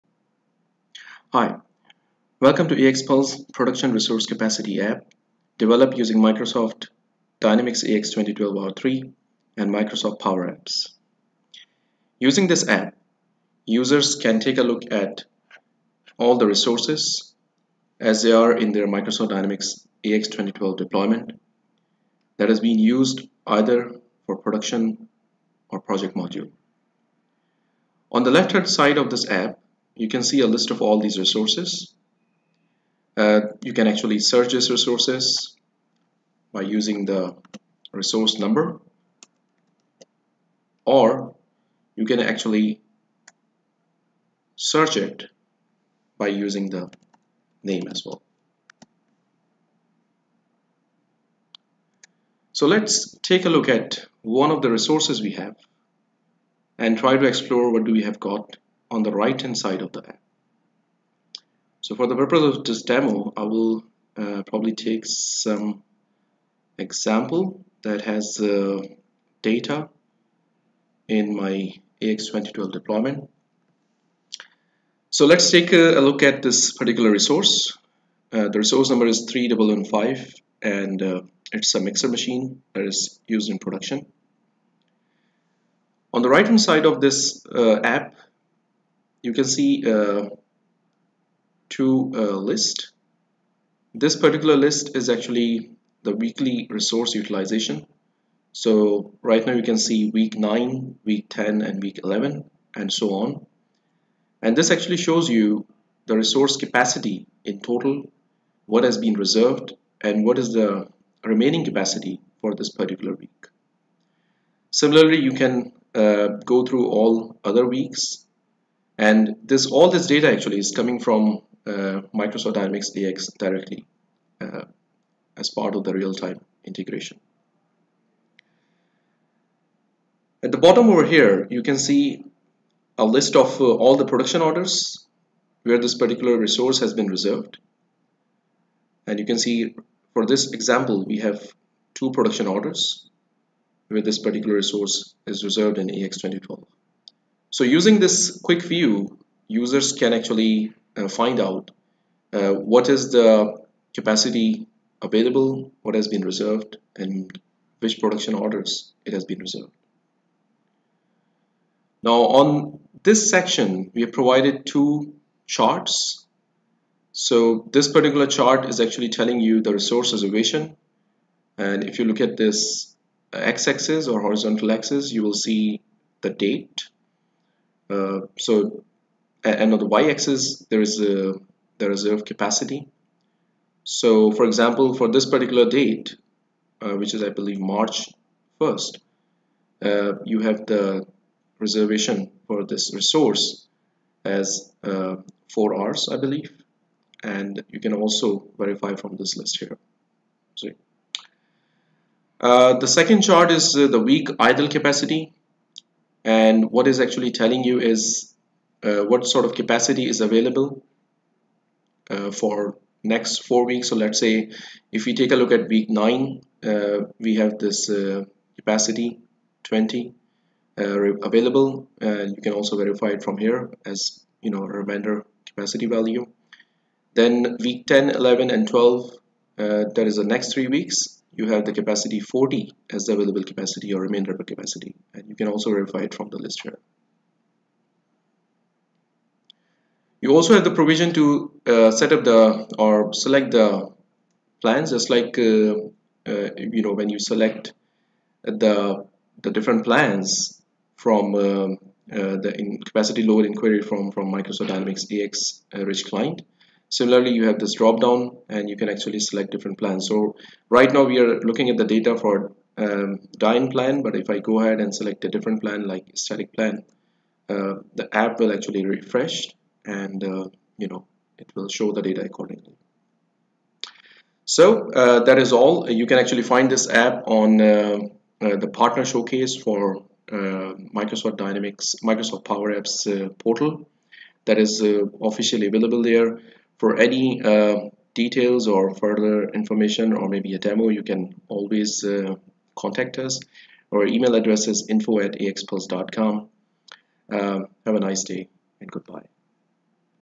AXPulse Production Resource Capacity
Production Resource Capacity app uses Microsoft Dynamics AX 2012 R3 resource capacity data to present it on a mobile or tablet machine using Microsoft Power Apps. You can view a list of resources, search and filter them to select a specific resource and view its total, reserved and remaining capacity information. The app also provides the information about production orders where the resource is reserved and graphs are also provided to present the daily and weekly resource capacity information. With this app, production managers and planners can easily access the capacity load information on their mobile or tablet devices without logging into Microsoft Dynamics AX 2012 R3.